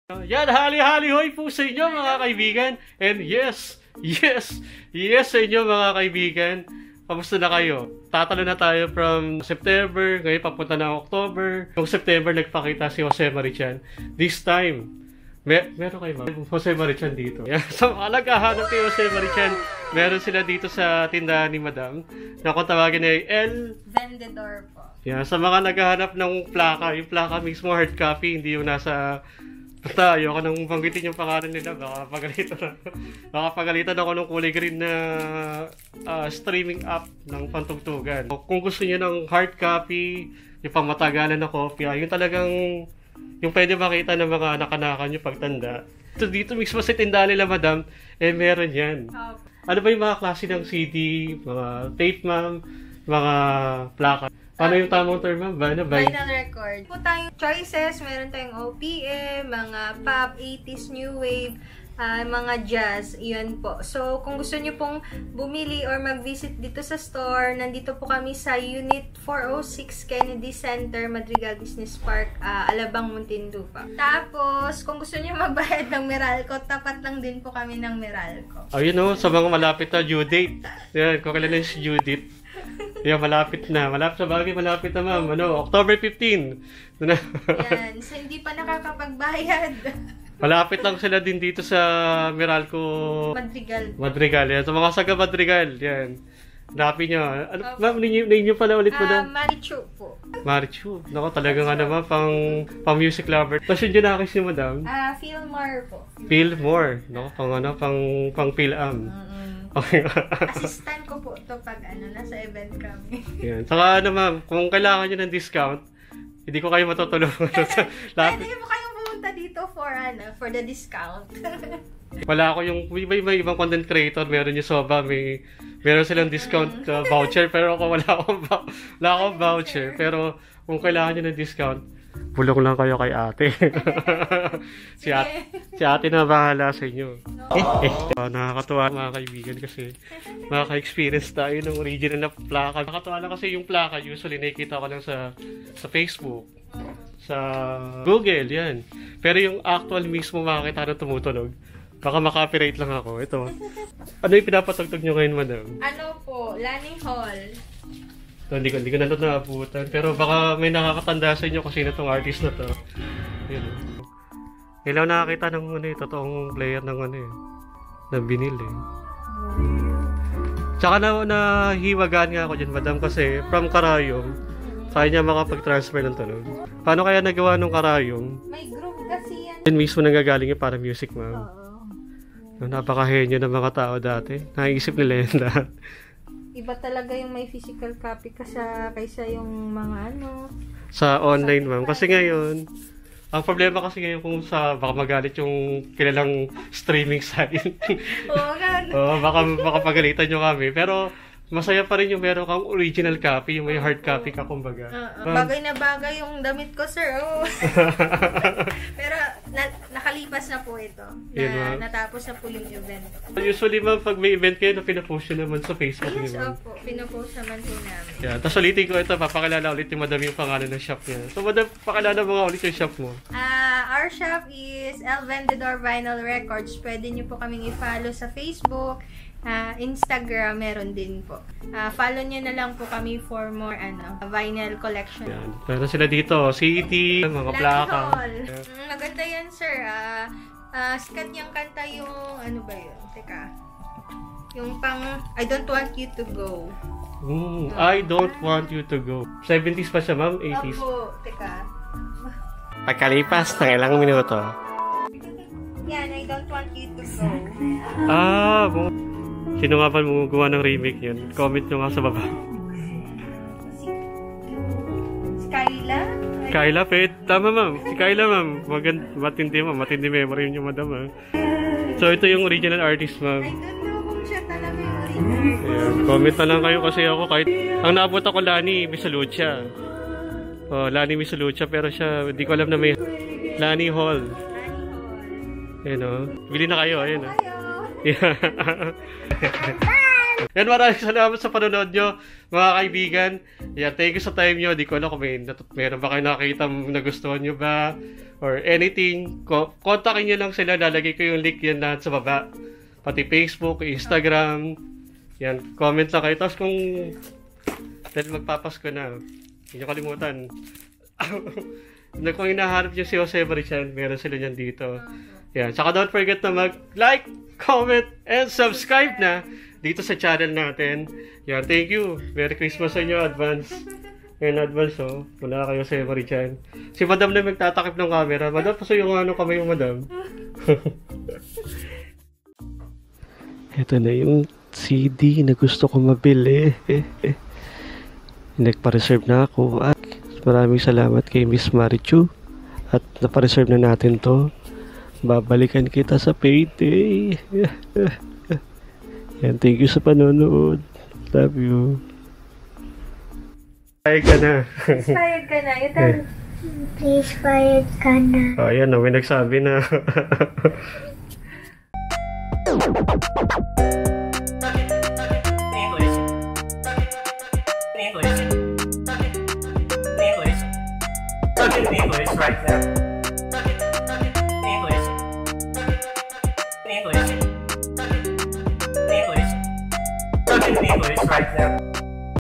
SECRET VINYL STORE SA MADRIGAL ALABANG
Hale Hale Hoy Mga Kaibigan!

Nag hahanap kaba ng mabibilihan ng Cassette Tape,
CD at Vinyl Records?

Punta na sa EL Vendedor Records

Suportahan natin ang mga pinoy
Youtubers :)

Kinig na sa Podcast:
Chats Nights with Jonn